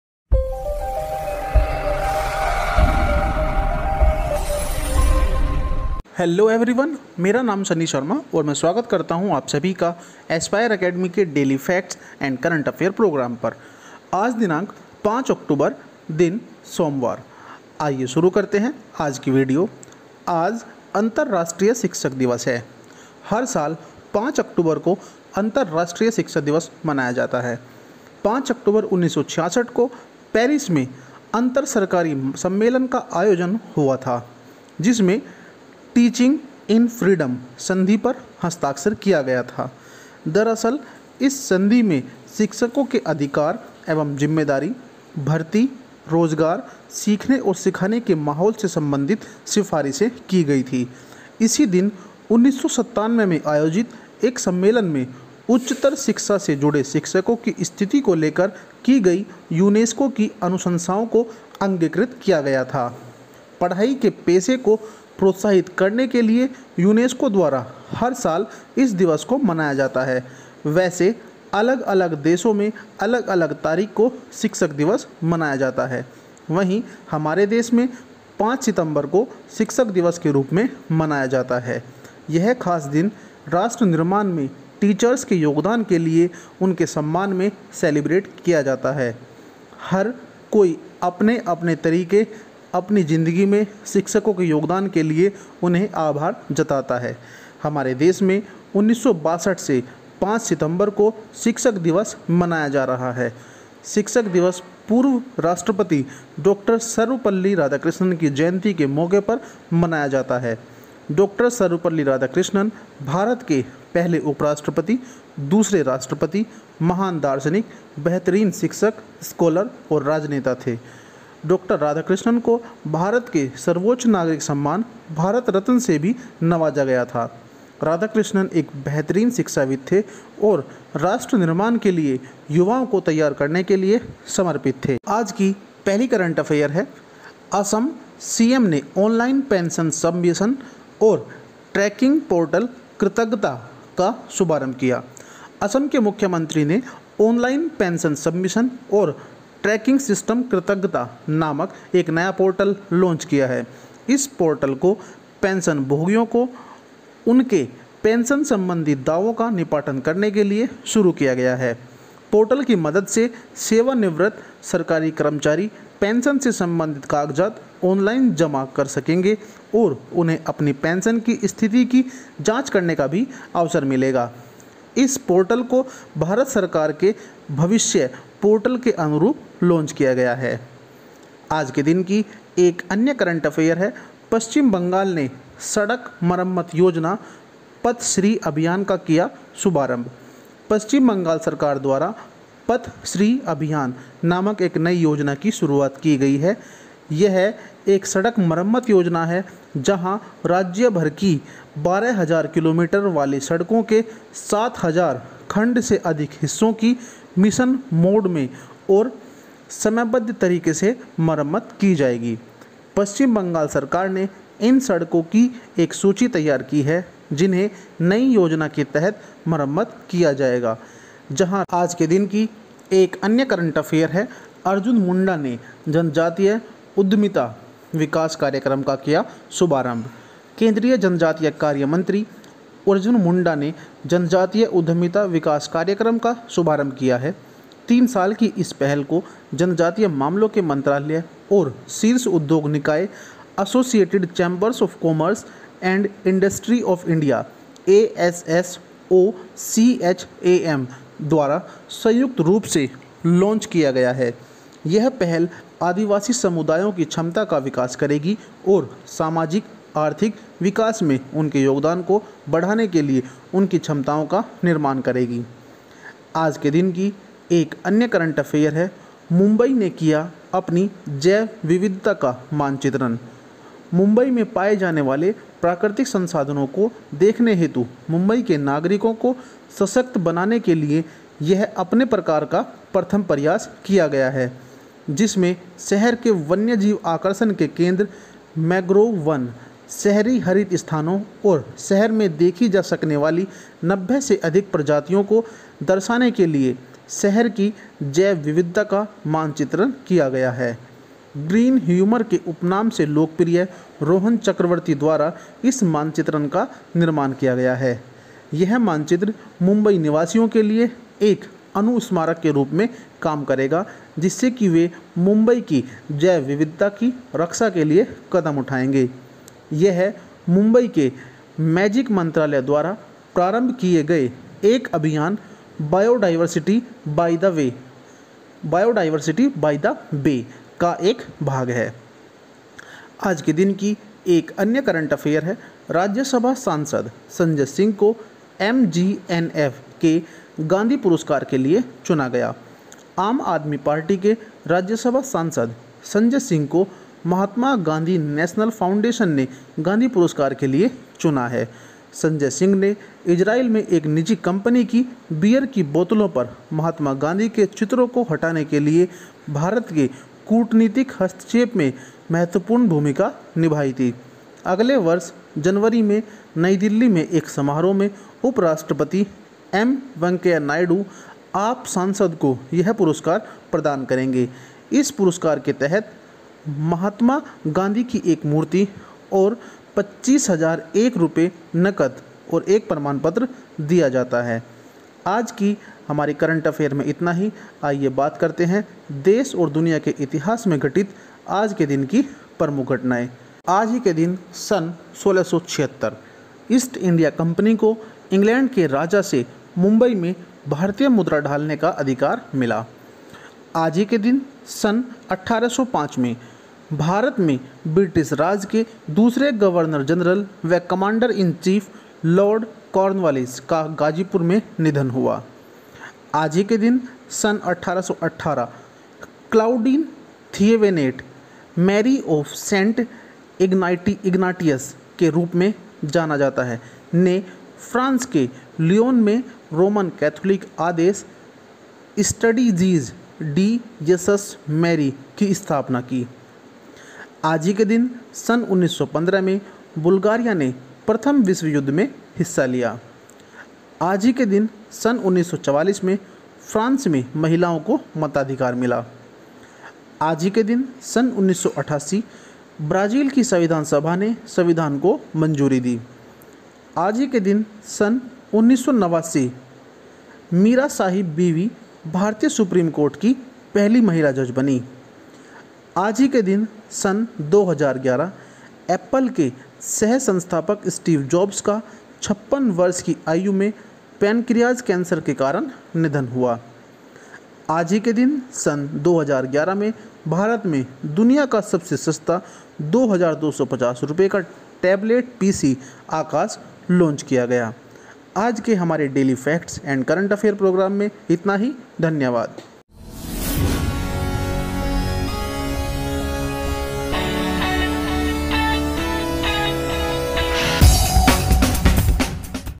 Daily Facts and Current Affairs 5 October 2020 by Sunny Sharma
In this video we are discussing about current affair and historical facts on 5 October by sunny sharma.